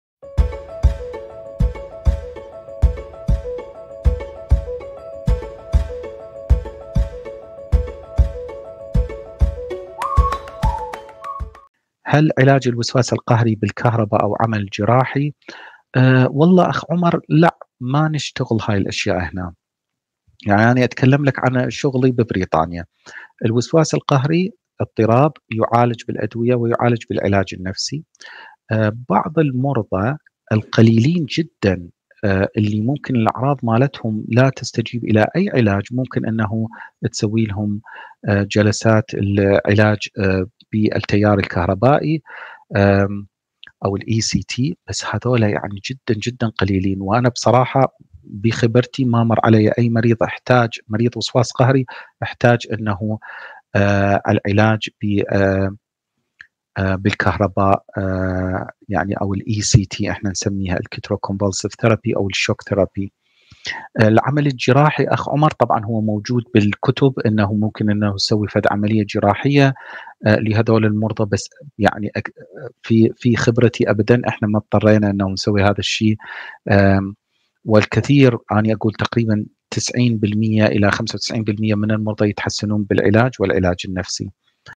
علاج الوسواس القهري بالصدمات الكهربائية
ياسر عبد المجيد حميد
استشاري الامراض النفسية
 عضو الكلية الملكية البريطانية لأطباء النفس
المملكة المتحدة
البريد الالكتروني:

صفحة الدكتورة بتول العيسى والدكتور ياسر حميد للصحة النفسية

An educational video in Arabic language
By Dr Yasir Hameed (Consultant Psychiatrist and Honorary Senior Lecturer)
UK